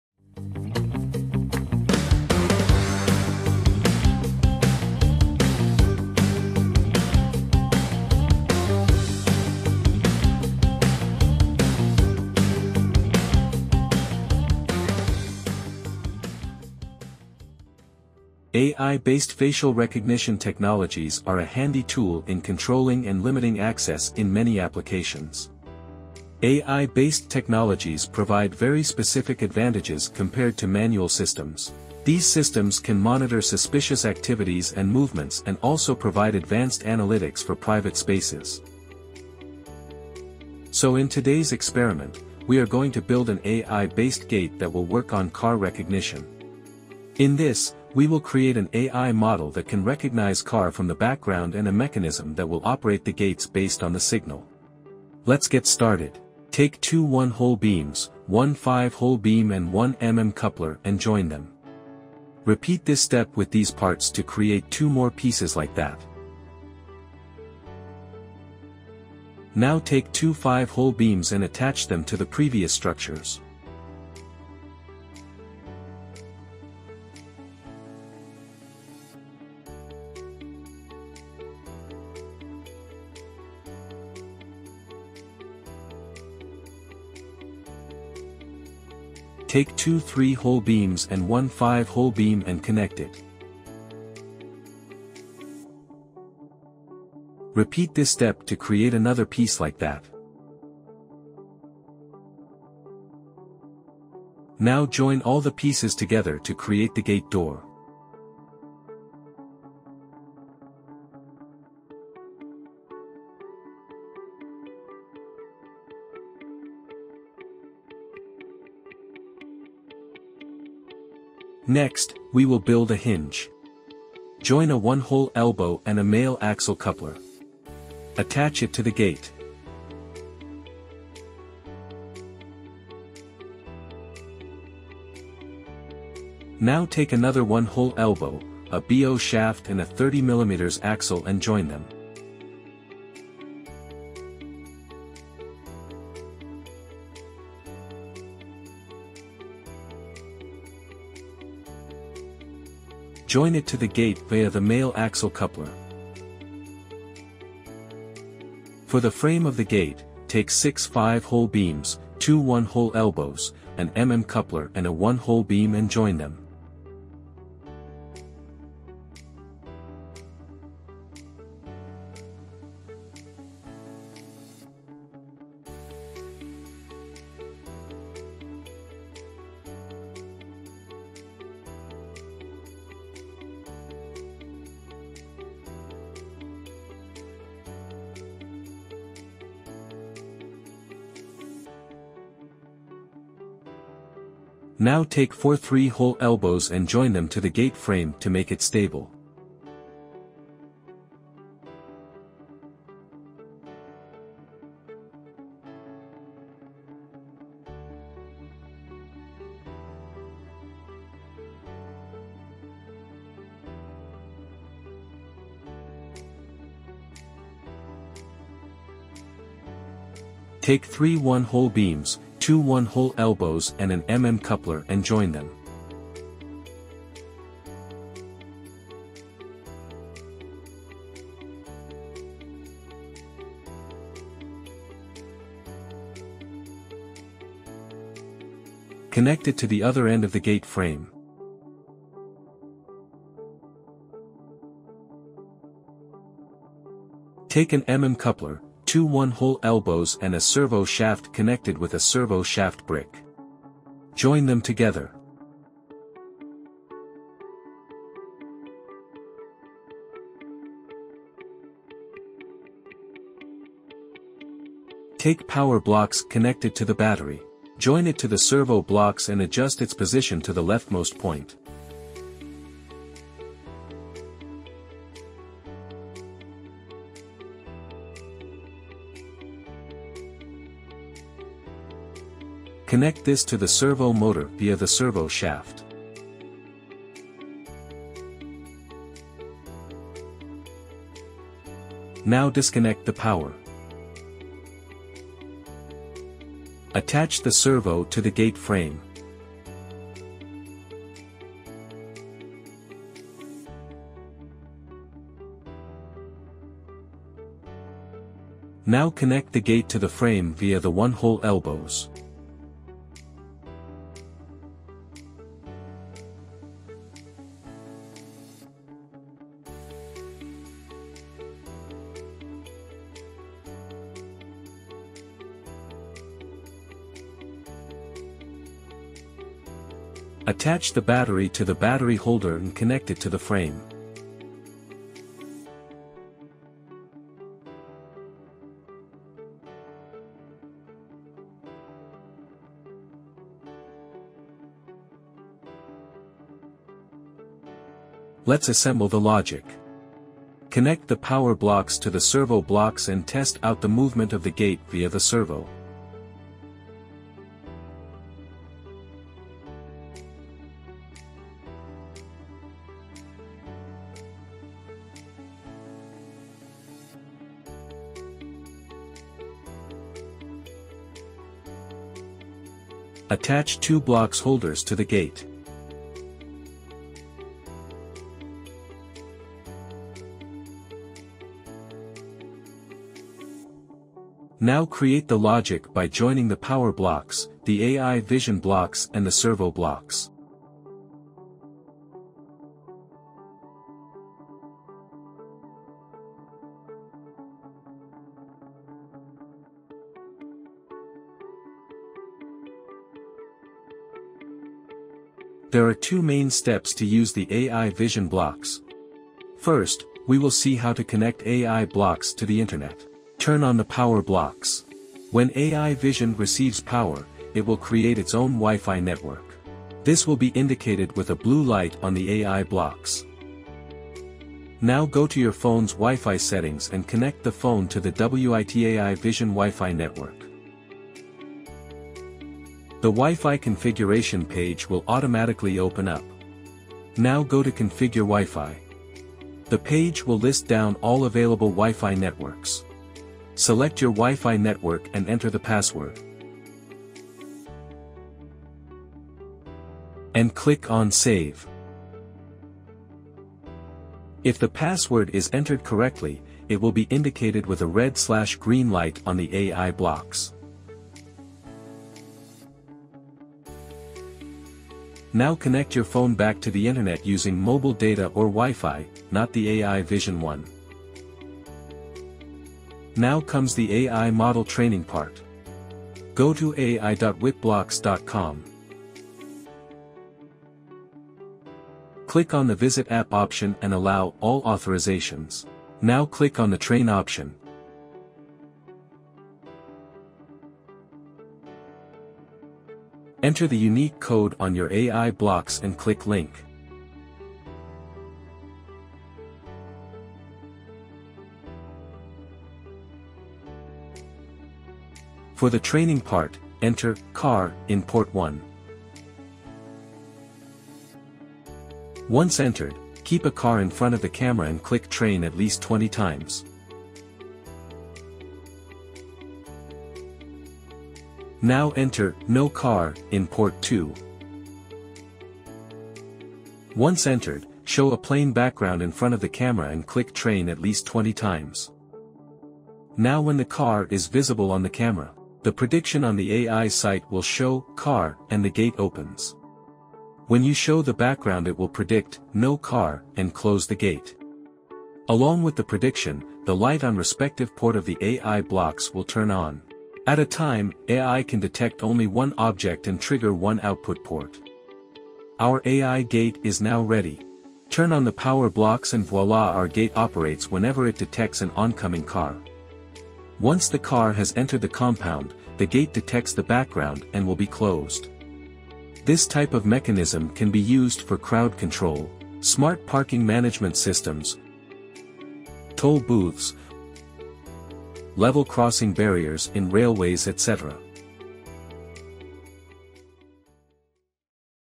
How to build an Automatic Gate using AI | AI Object Detection
Heyy Makers
Presenting the brand new AI-Based Automatic Gate. Smart and can work for long durations without a break.
No need to manually open or close the gates when you have AI based smart detection enabled.

This Gate can also be trained to detect people in a society to allow for quick access

So what are you waiting for? Take out your Witbricks and Plug n Play WitBlox parts and get started..

Happy Building

New to the WitBlox World.???

Already have the electronics and just need the bricks. We have got you covered.